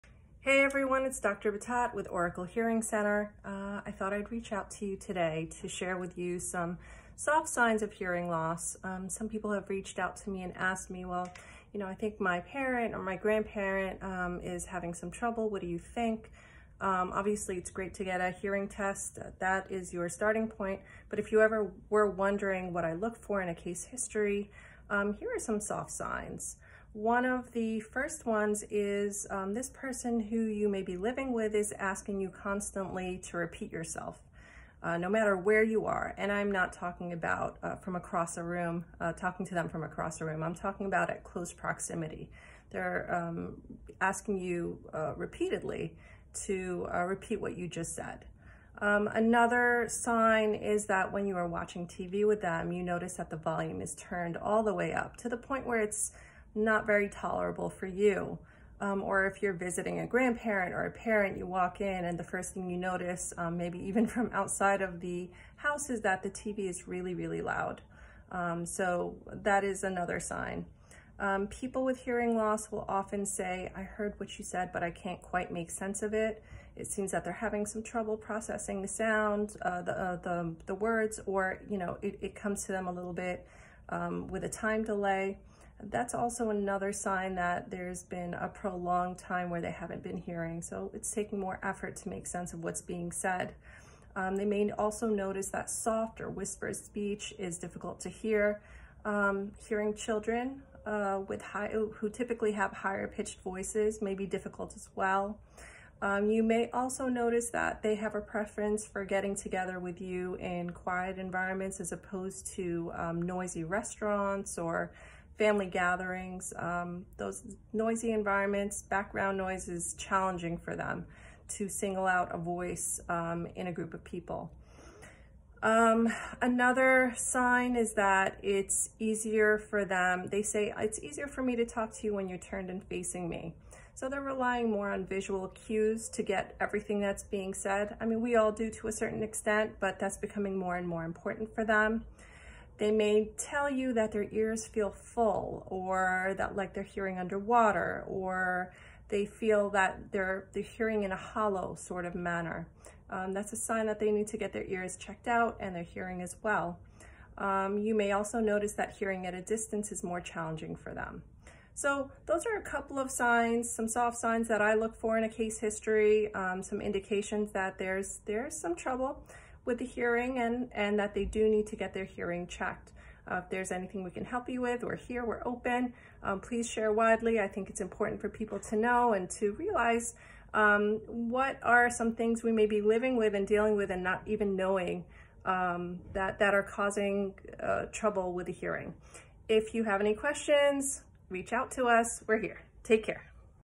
Soft Signs of Hearing Loss
If you’ve ever wondered whether or not you or someone close to you has a hearing loss, here are some soft signs.